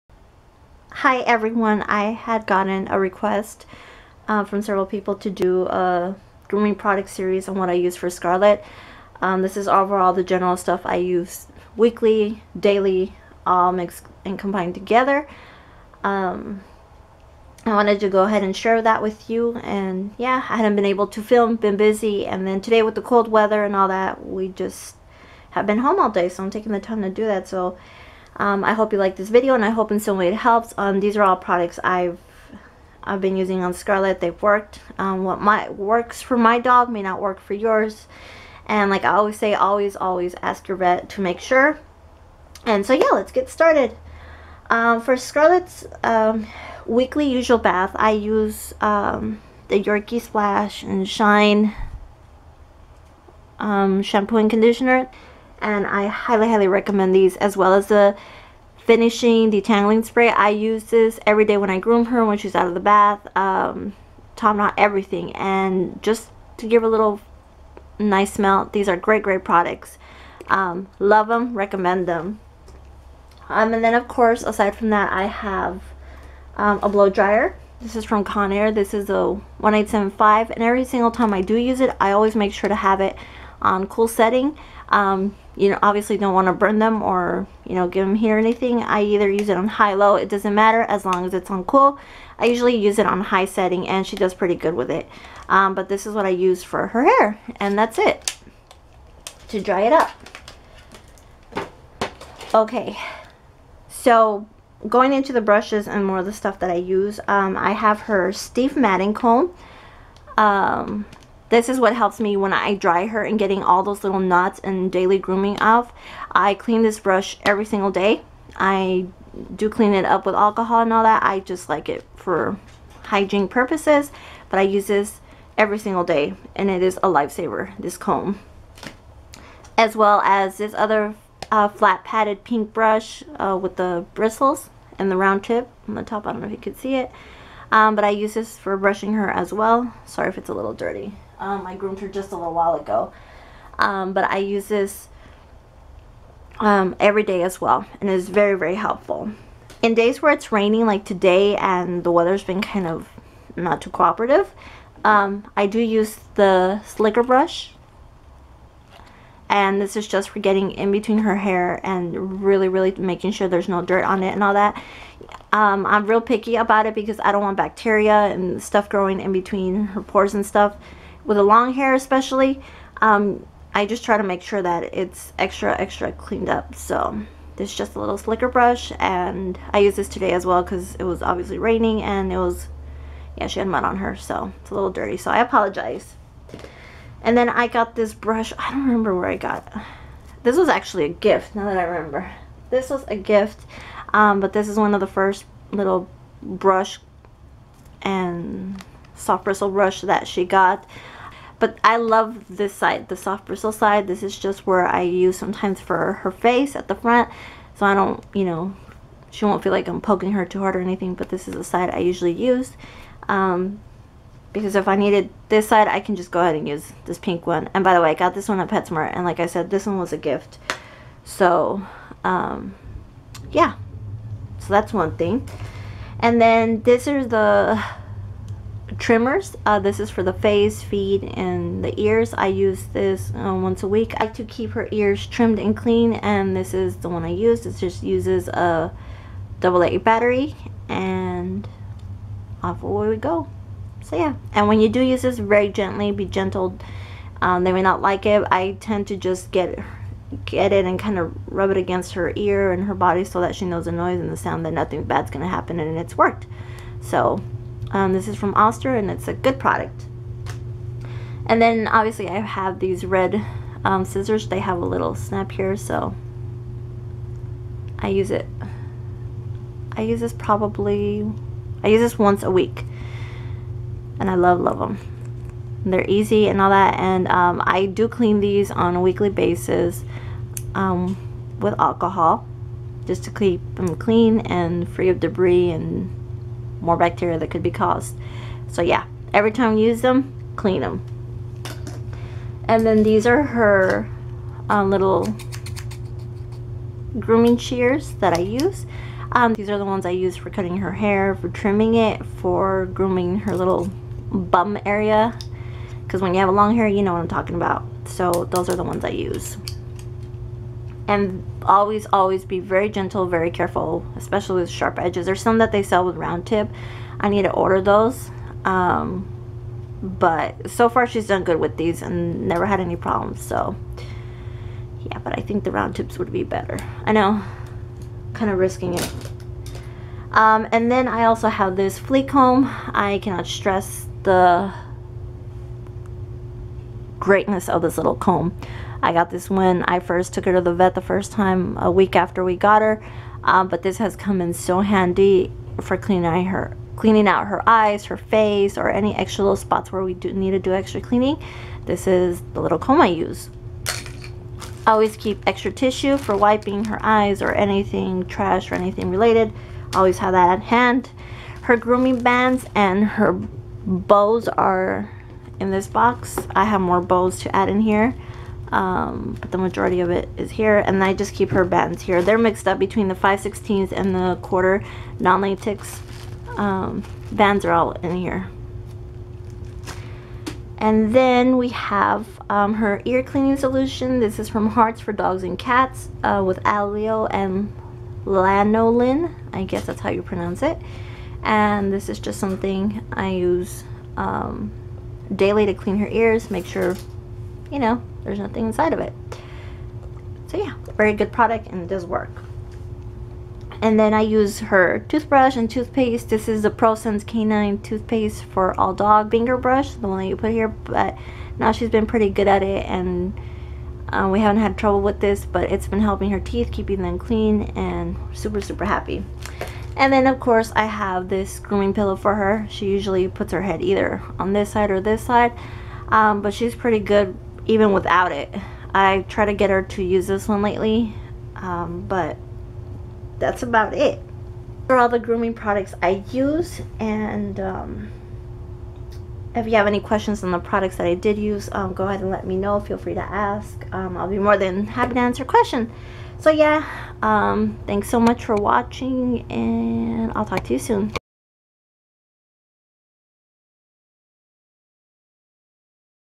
Grooming Products for My Yorkie
This was a requested video on the grooming products used for my dog Scarlett.